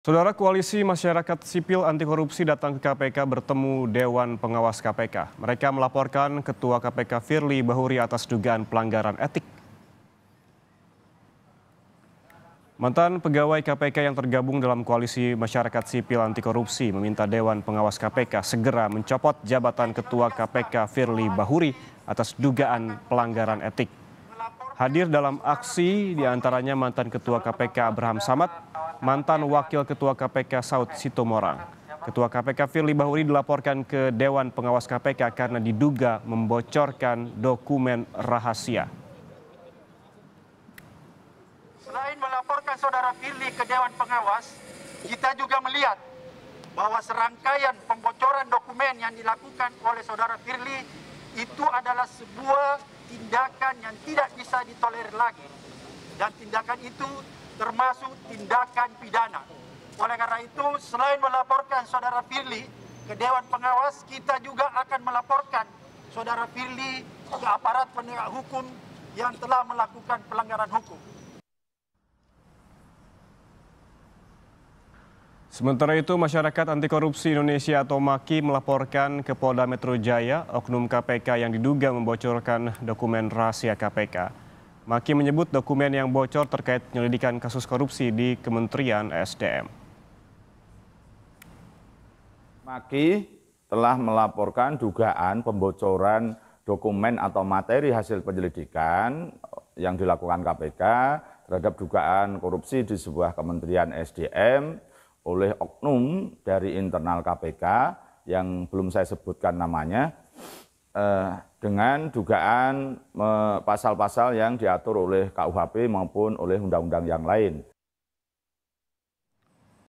MAKI Minta Dewas Copot Wakil Ketua KPK atas Dugaan Pelanggaran Etik
KOMPAS.TV - Koalisi Masyarakat Sipil Anti Korupsi (MAKI) datang ke KPK bertemu Dewan Pengawas KPK. Mereka melaporkan Ketua KPK Firli Bahuri atas dugaan pelanggaran etik.

Mantan pegawai KPK yang tergabung dalam koalisi MAKI meminta Dewan Pengawas KPK segera mencopot jabatan ketua KPK Firli Bahuri atas dugaan pelanggaran etik.

Hadir dalam aksi di antaranya mantan Ketua KPK Abraham Samad, mantan Wakil Ketua KPK Saut Sitomorang.

Ketua KPK Firli Bahuri dilaporkan ke Dewan Pengawas KPK karena diduga membocorkan dokumen rahasia.

Sementara itu, MAKI melaporkan ke Polda Metro Jaya oknum KPK yang diduga membocorkan dokumen rahasia KPK.

MAKI menyebut, dokumen yang bocor terkait penyelidikan kasus korupsi di Kementerian ESDM.

Baca Juga Akses Dicabut, Brigjen Endar Priantoro Tak Bisa Masuk Gedung KPK di